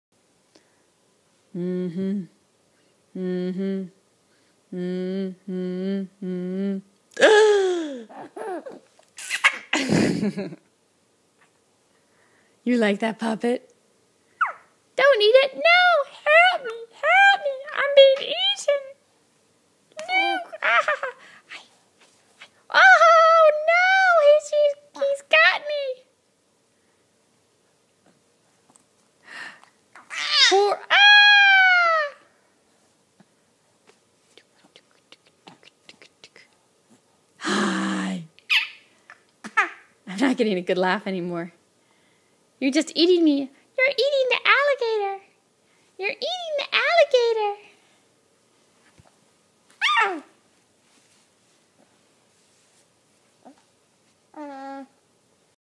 Laughing Luke likes lizards
Fun with the alligator puppet.